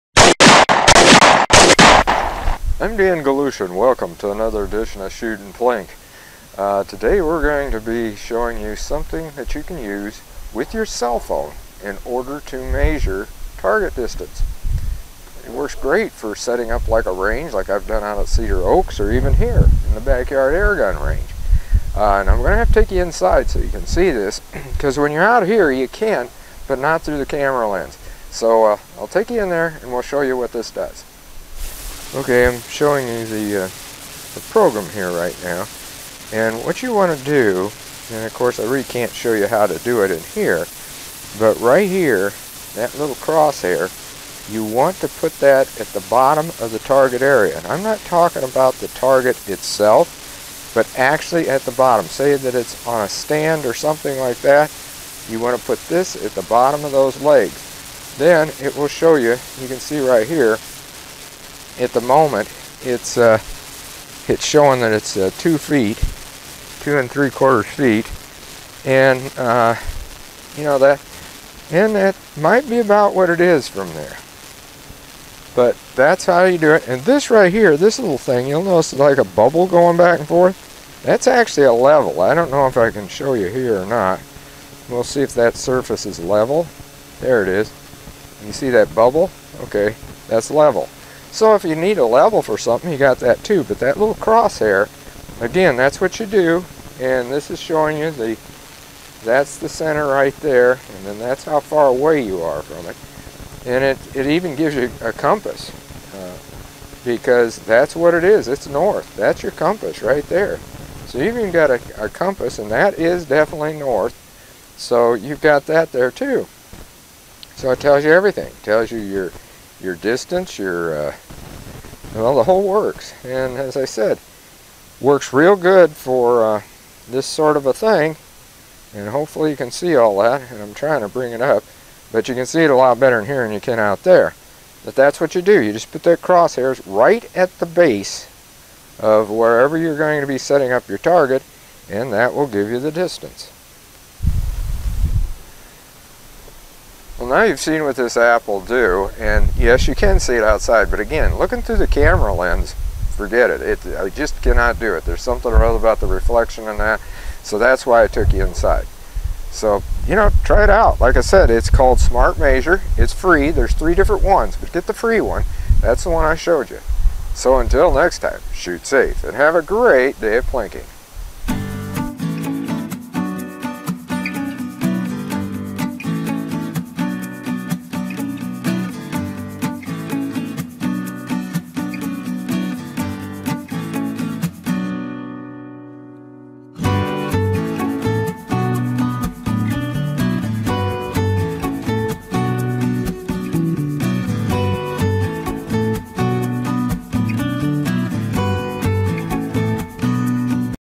Using Your Cellphone for a Rangefinder
If you are looking for a quick, cheap and easy rangefinder, level and compass try the Smart Measure app on your cellphone and tablet.  There are 3, but here we are talking about the free version.  Works great for setting up a shooting range for target distances.